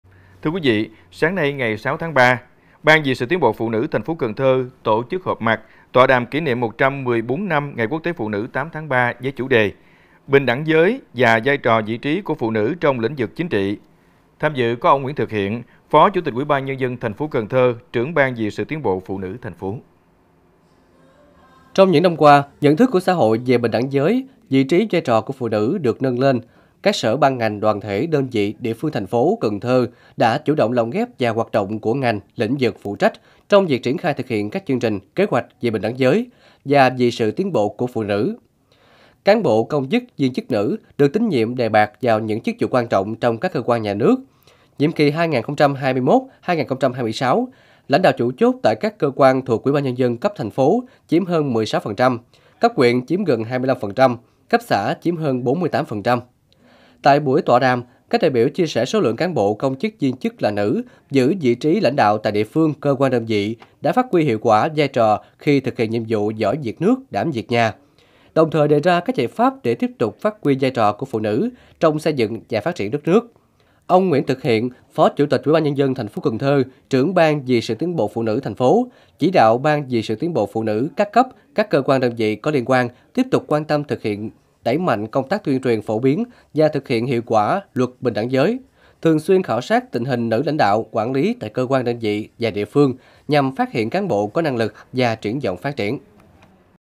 Họp mặt, tọa đàm kỷ niệm ngày Quốc tế Phụ nữ | Cần Thơ TV
Sáng 6/3, Ban vì sự tiến bộ phụ nữ thành phố Cần Thơ tổ chức họp mặt, tọa đàm kỷ niệm 114 năm ngày Quốc tế Phụ nữ 8/3 với chủ đề “Bình đẳng giới và vai trò, vị trí của phụ nữ trong lĩnh vực chính trị”. Tham dự có ông Nguyễn Thực Hiện - Phó Chủ tịch UBND TP. Cần Thơ, Trưởng Ban vì sự tiến bộ phụ nữ thành phố.